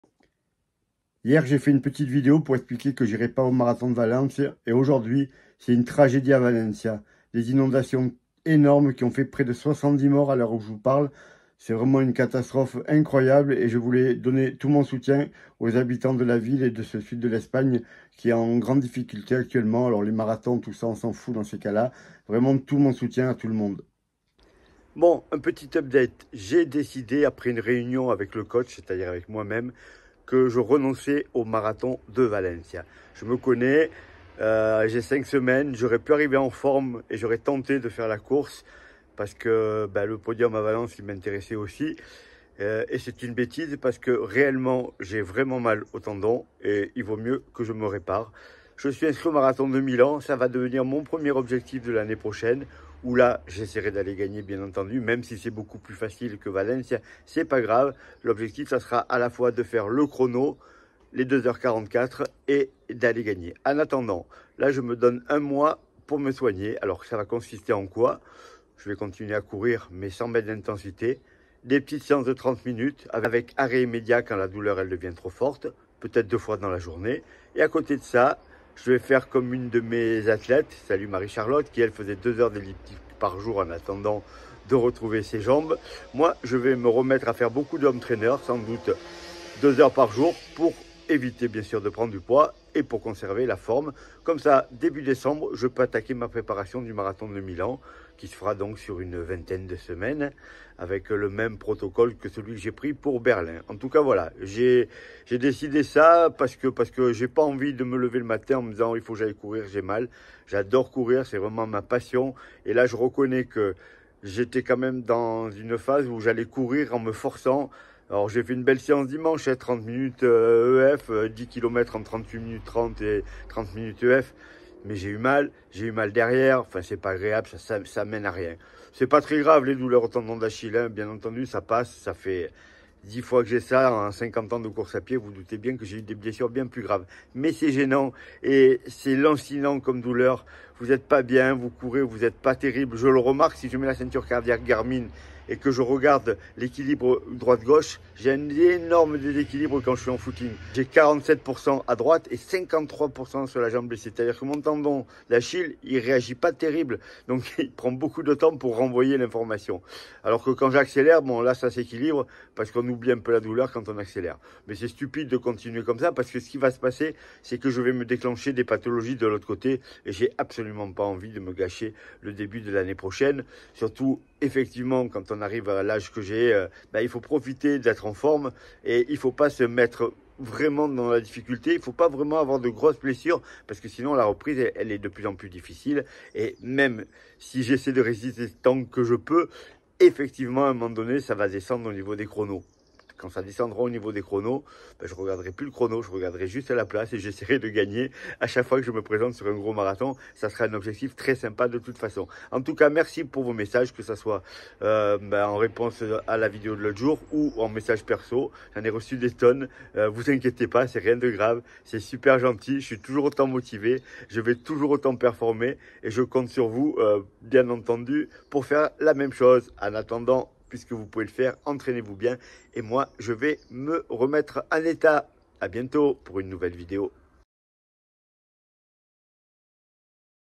Soutien aux habitants de Valencia ! Ma vidéo tourné hier n'a plus de sens hélas !
Hier j’ai tourné une petite vidéo pour dire que je n’irai pas à Valencia pour le Marathon.. mais je l’avais pas encore mis en ligne..

Elle paraît bien aujourd’hui, quand je vois la catastrophe qui est arrivé dans la ville ..

Tout mon soutien aux habitants du sud de l’Espagne …😔